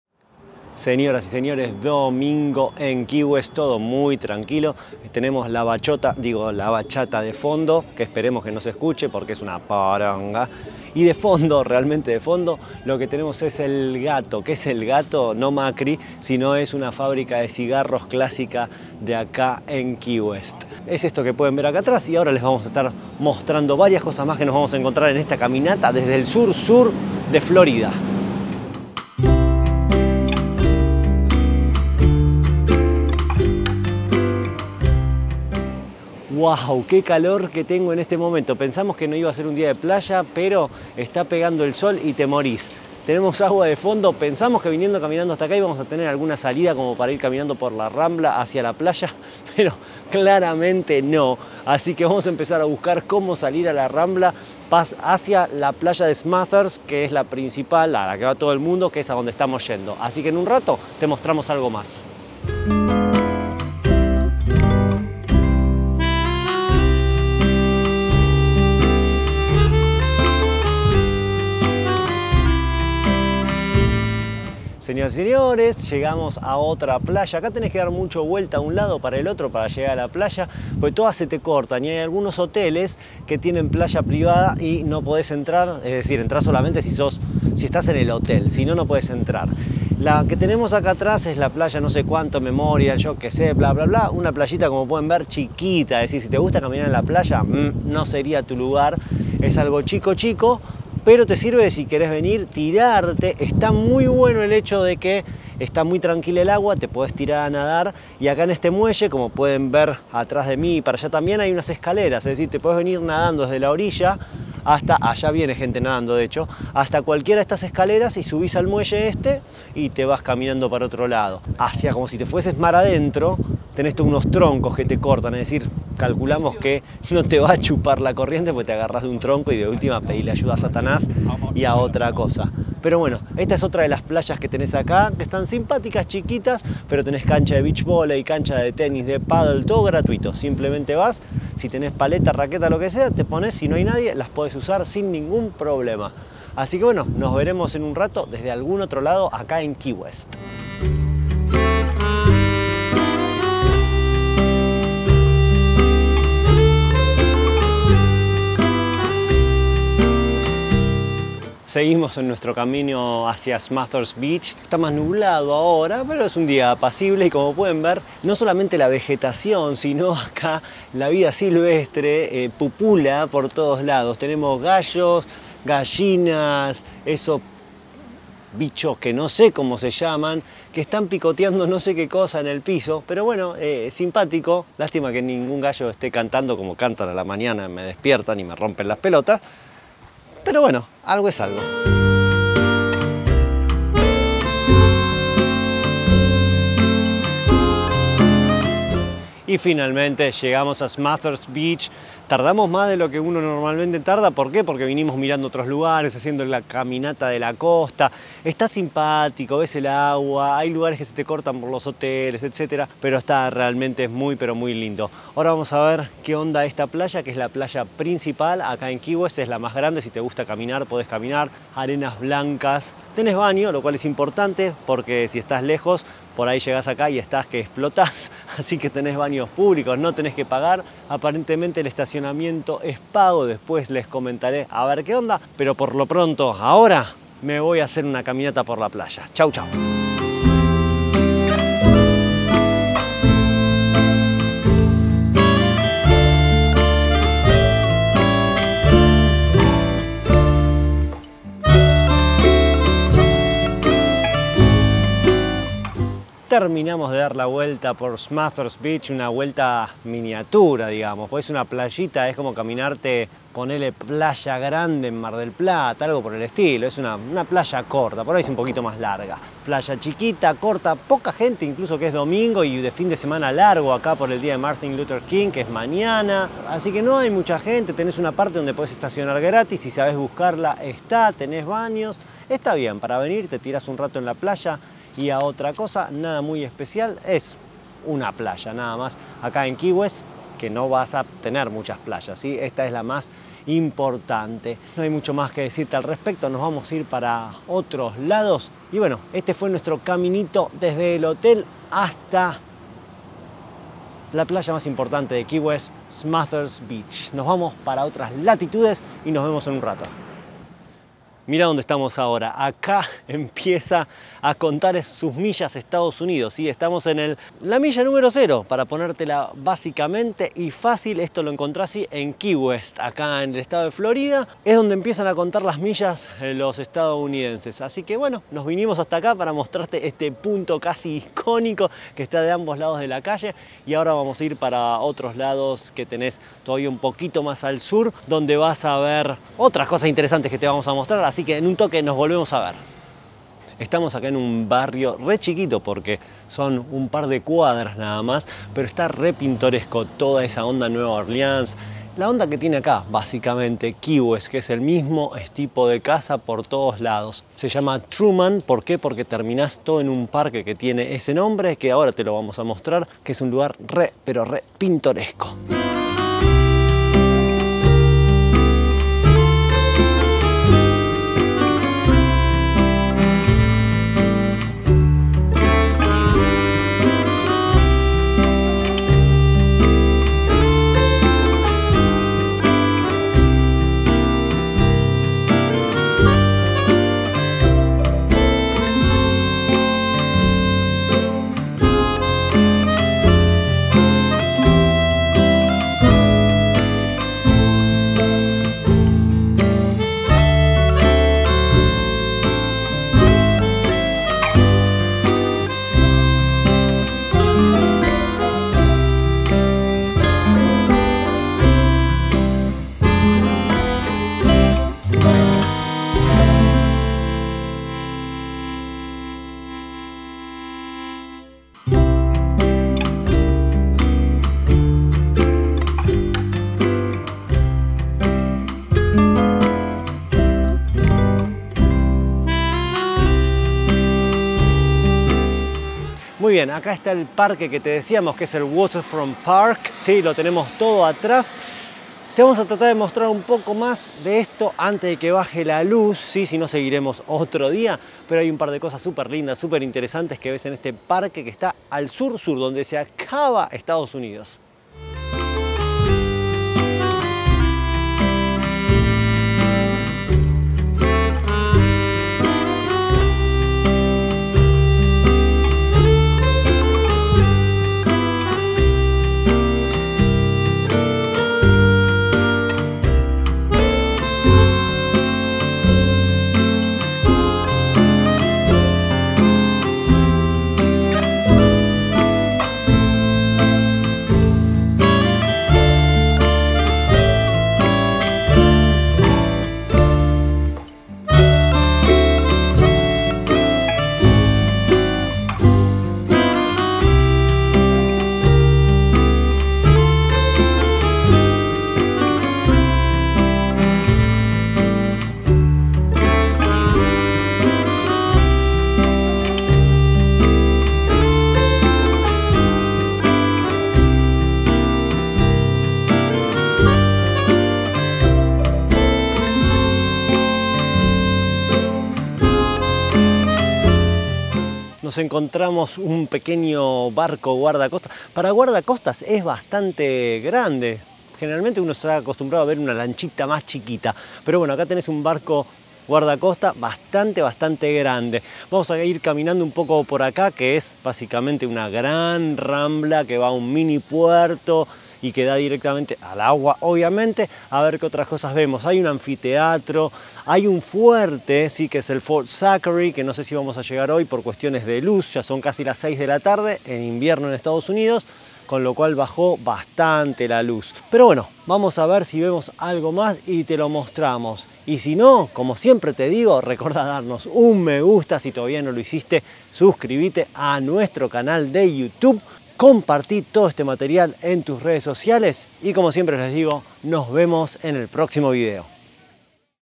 Recorriendo Key West #1 (enero 2021)
¿Nunca estuviste en Key West? No te preocupes!!! Acá te mostramos muchas de las cosas que podés ver allí como para que te las agendes y tengas en cuenta cuando tengas la suerte de visitar el punto más al sur de los EE.UU.